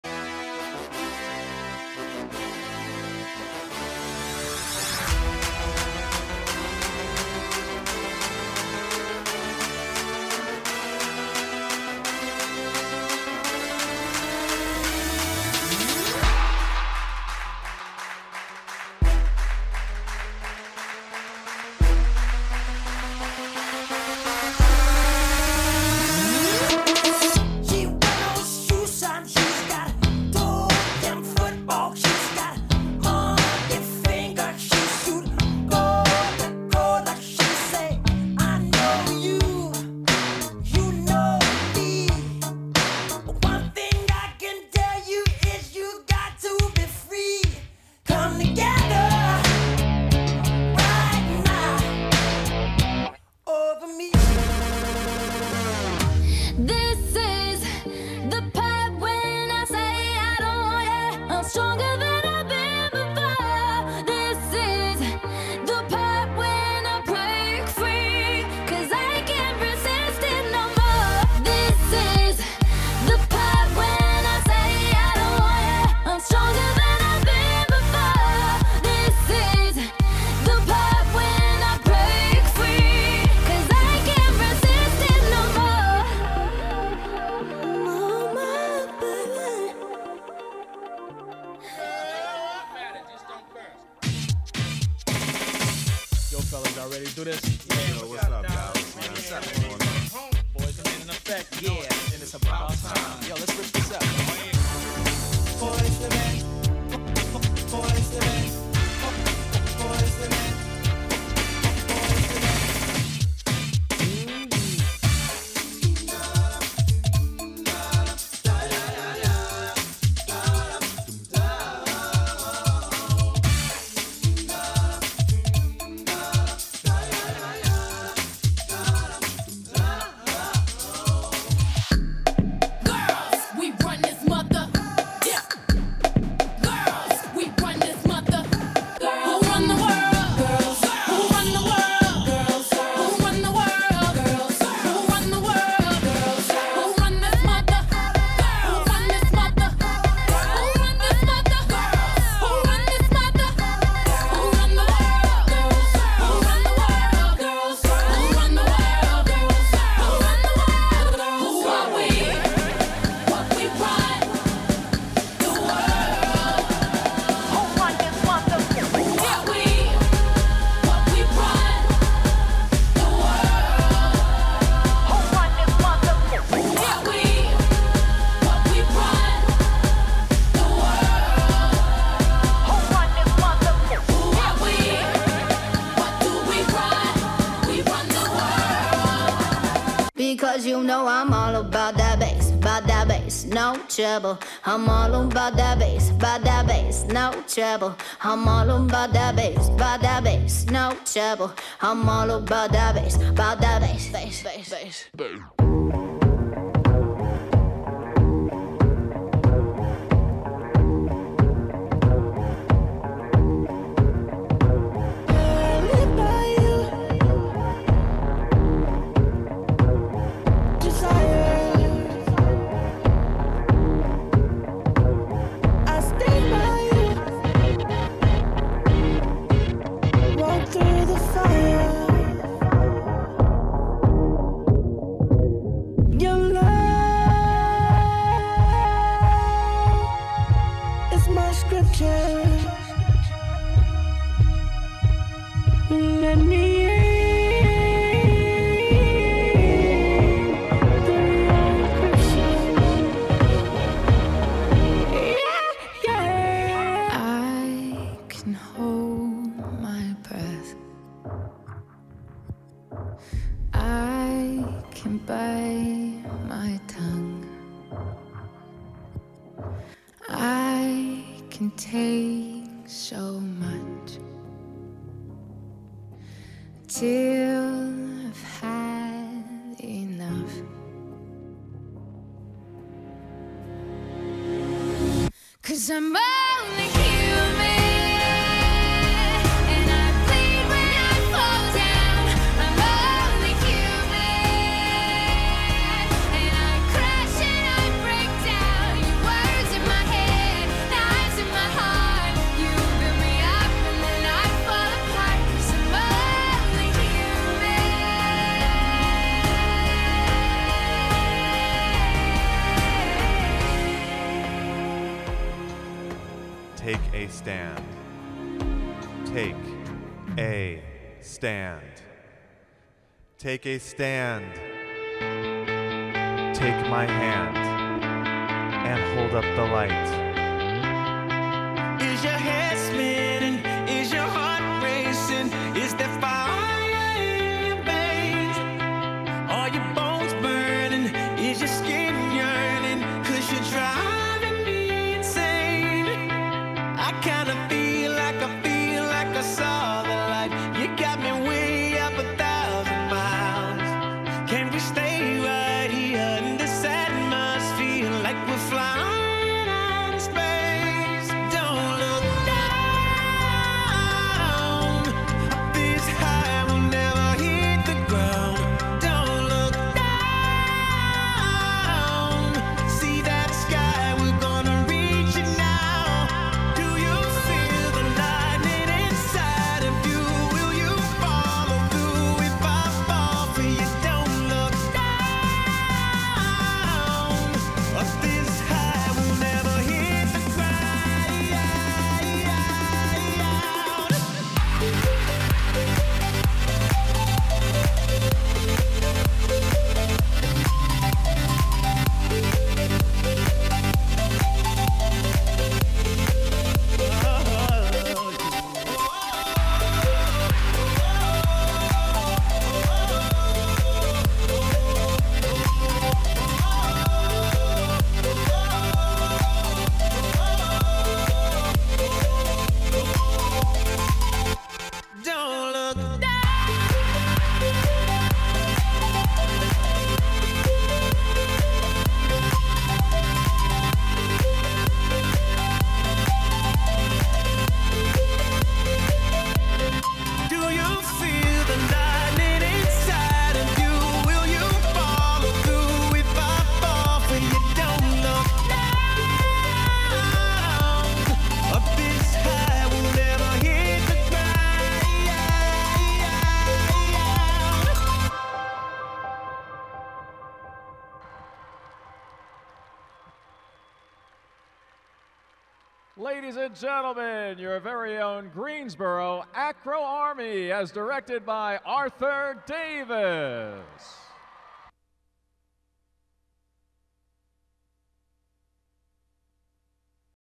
U.S. Acro Elite Gymnasts featuring Acro Army
A special performance by U.S. acrobatic gymnastics athletes featuring Acro Army. This routine was performed at the 2015 USA Gymnastics Championships in Greensboro, N.C. on June 27, 2015.